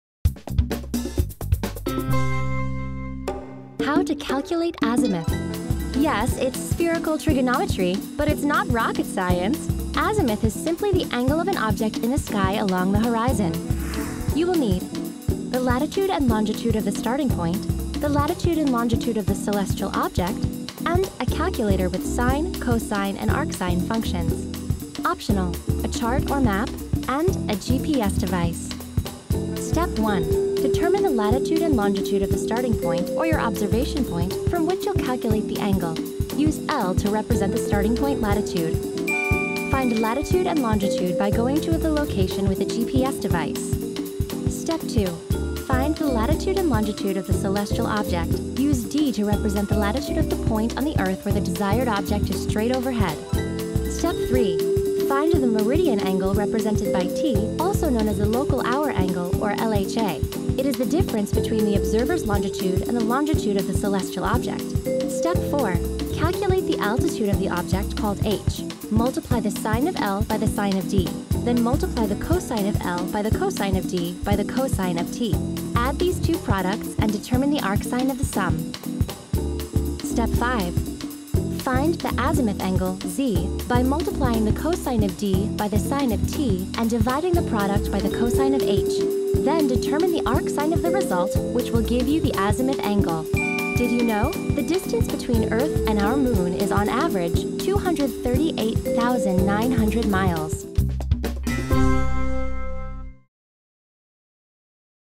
How to Calculate Azimuth
Yes, it's spherical trigonometry – but it's not rocket science! Azimuth is simply the angle of an object in the sky along the horizon.

Step 1: Determine latitude and longitude
Determine the latitude and longitude of the starting point, or observation point, from which you'll calculate the angle. Use L to represent the starting point latitude.

Tip
Find latitude and longitude by going to the location with a GPS device.

Step 2: Find object's coordinates
Find the latitude and longitude of the celestial object. Use D to represent the latitude of the point on the earth where the desired object is straight overhead.

Step 3: Find t
Find the meridian angle, represented by t, also known as the local hour angle – or LHA. It is the difference between the observer's longitude and the longitude of the celestial object.

Step 4: Calculate the altitude
Calculate the altitude of the object, called H. Multiply the sine of L by the sine of D. Then multiply the cosine of L by the cosine of D, by the cosine of t. Add these two products and determine the arcsine of the sum.

Step 5: Compute azimuth
Find the azimuth angle, Z, by multiplying the cosine of D by the sine of t, and dividing the product by the cosine of H. Then determine the arcsine of the result, which will give you the azimuth angle.

The distance between Earth and our moon is, on average, 238,900 miles.